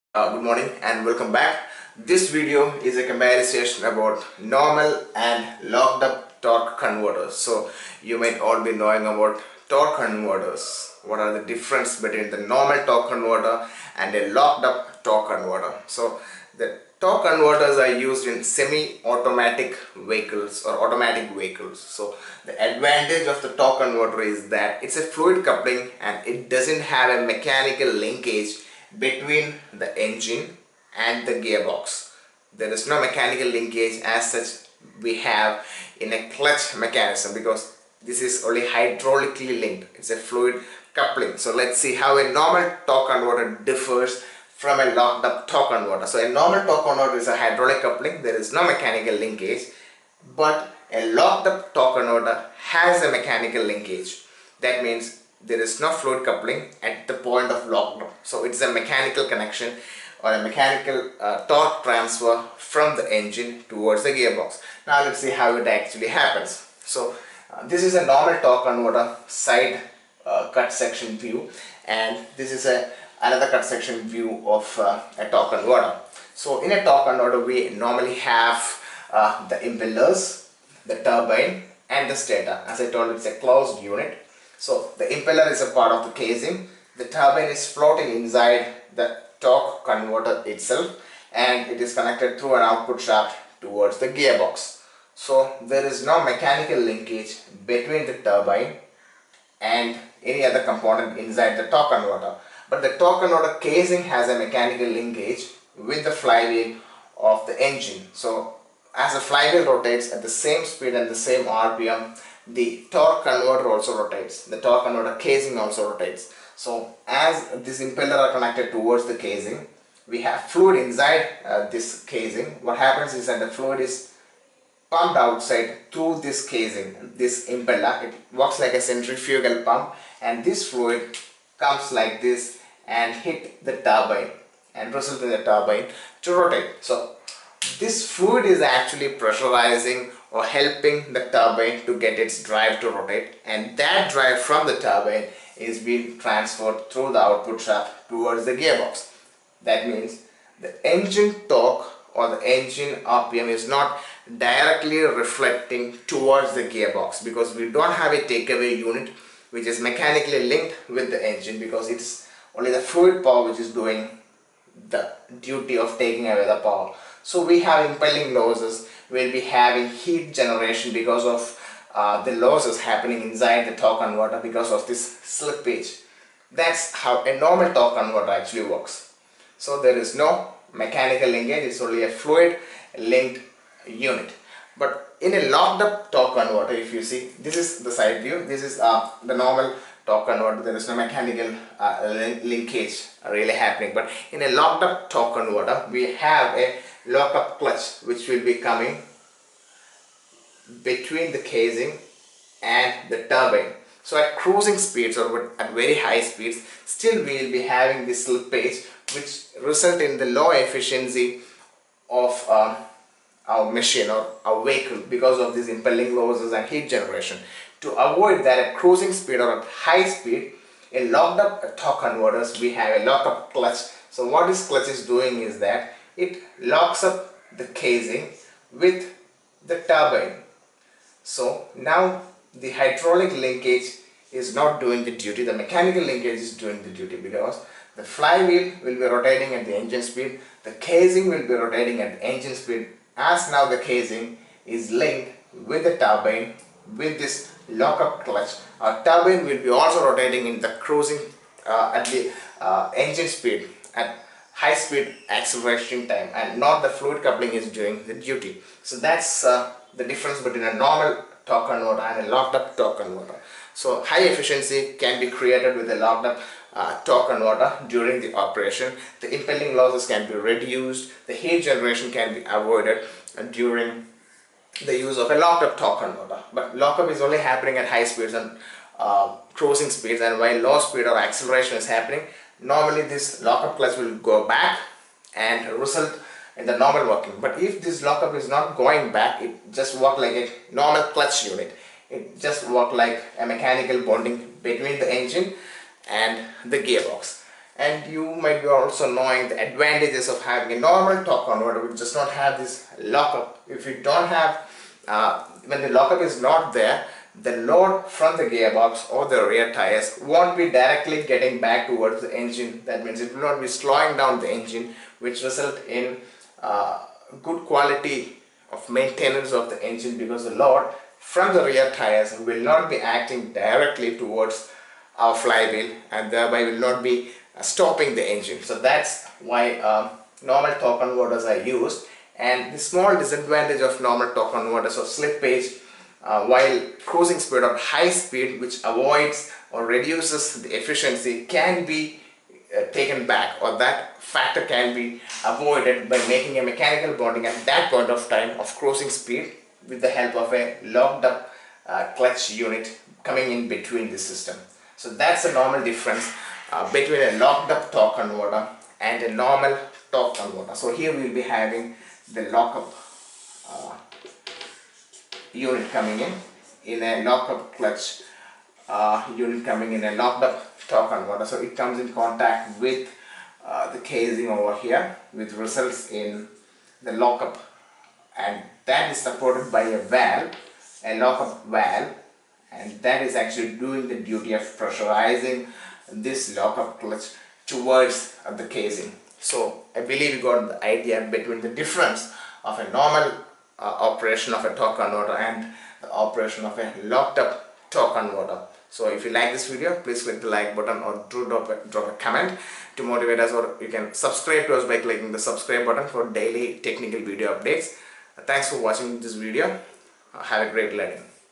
Locked up Vs Non Lock-up torque converters. ✔
Torque converter is a fluid coupling, but the lock up torque converter is a mechanical coupling. This video compares between a lockup torque converter and a non lockup torque converter.

Please click the like button or drop a comment if this was useful.
Thanks for your support and motivation.